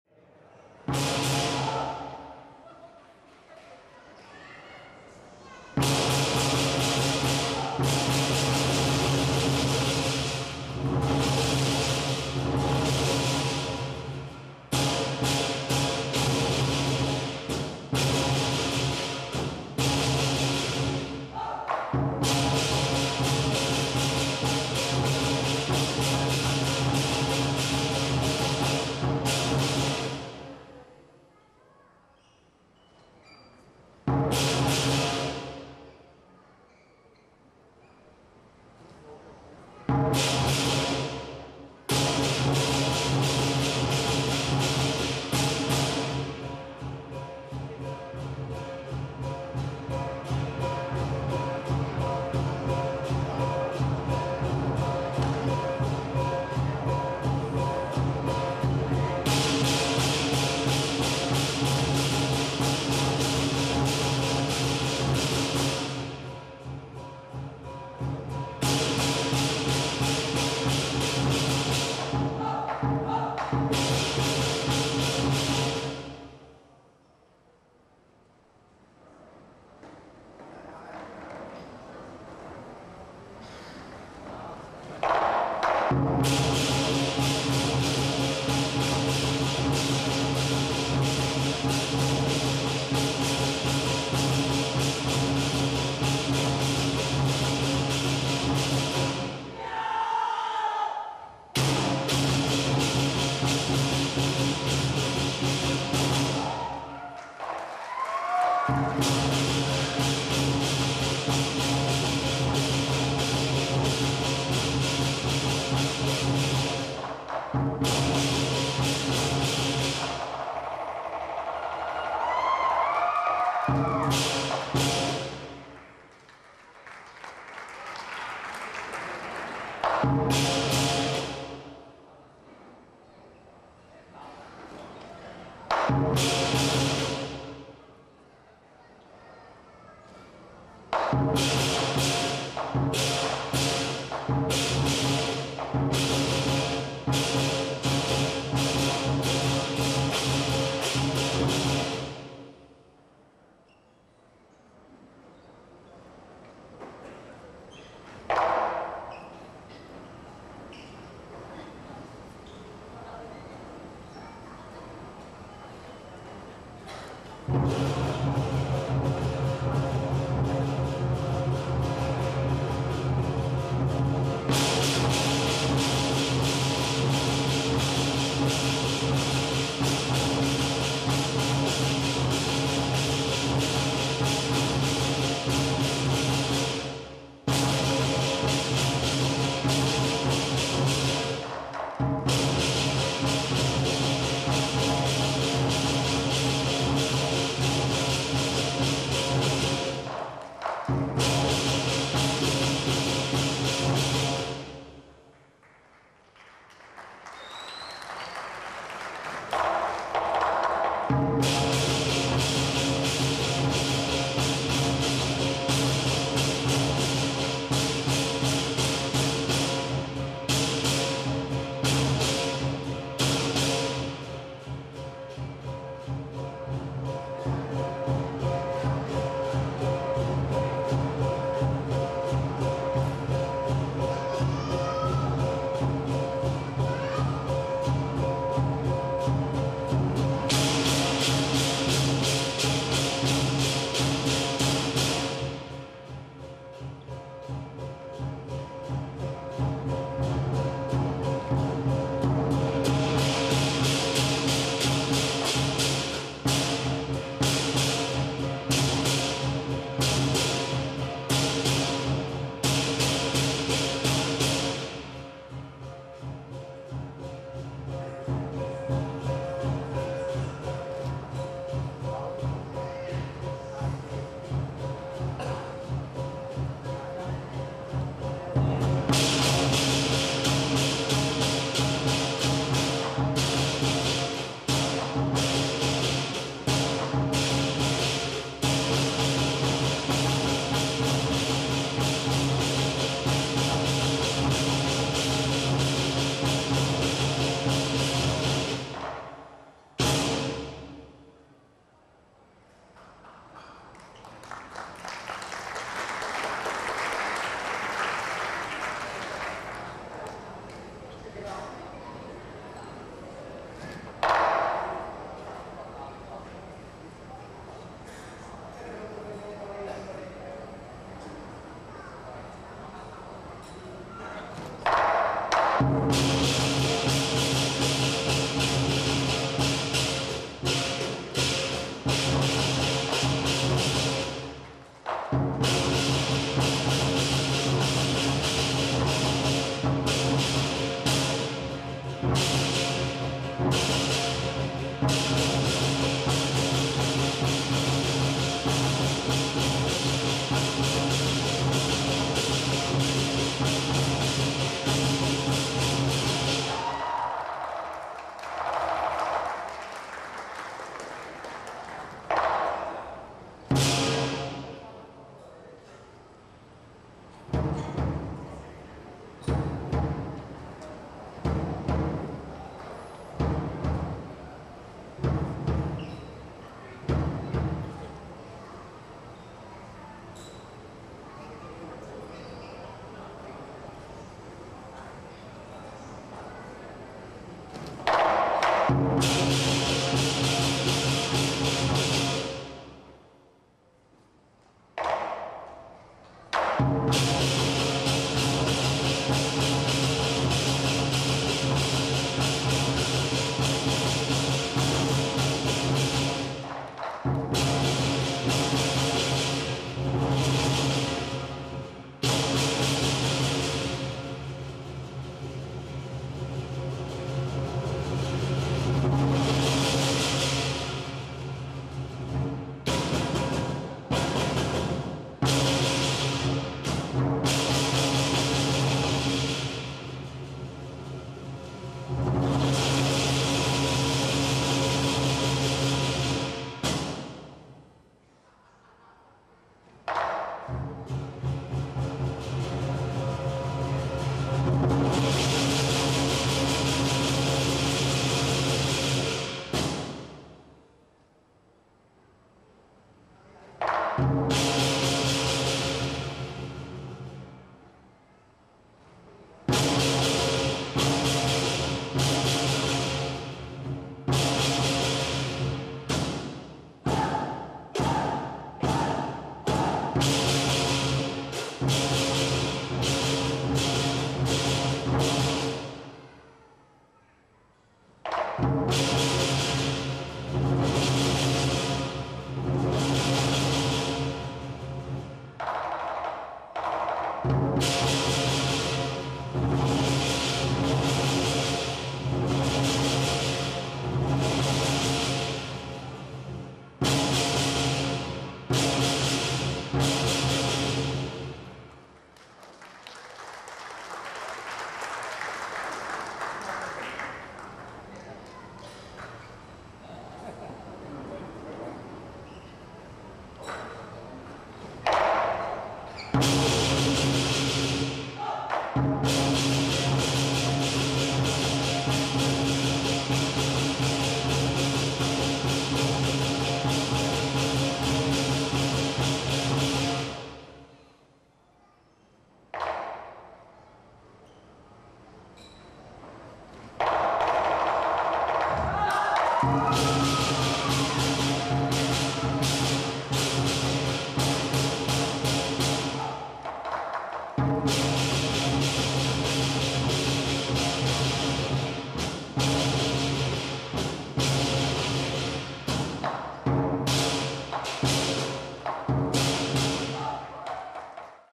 马来西亚麻坡关聖宫龙狮团
2010年马来西亚第六届全国龙狮锦标赛(DVD)